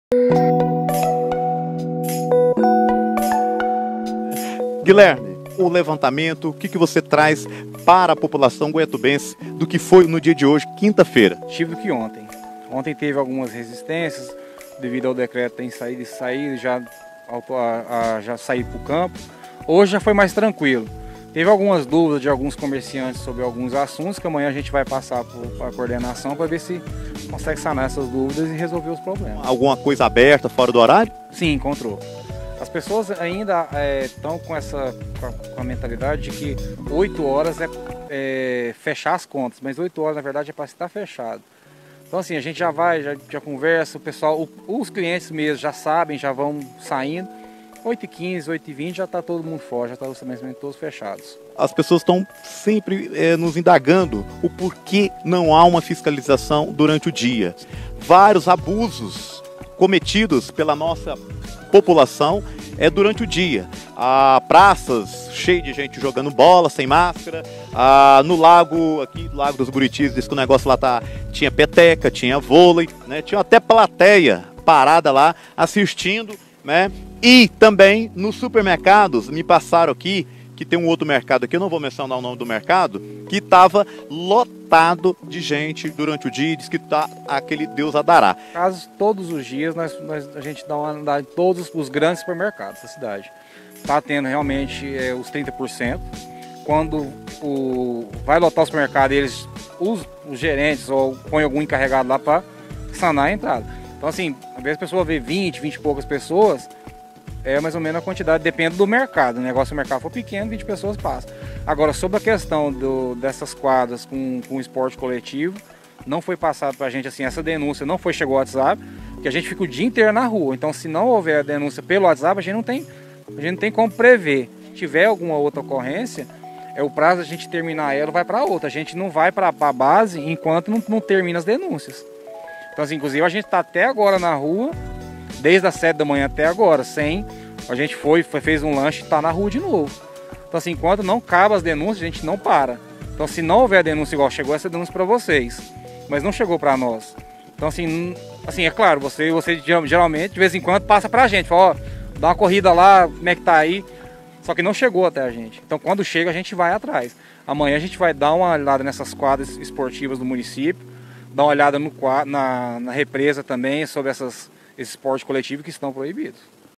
COVID-19 GOIATUBA HMG, ENTREVISTA COM FISCAL DA VIGILÂNCIA SANITÁRIA 04/03
Guilherme Bonifácio agente da vigilância sanitária informou que o trabalho neste última Quinta (04) foi tranquilo, e as pessoas estão mais conscientes,  só houve algumas dúvidas de alguns comerciantes que será levada até a coordenação e repassada aos mesmos.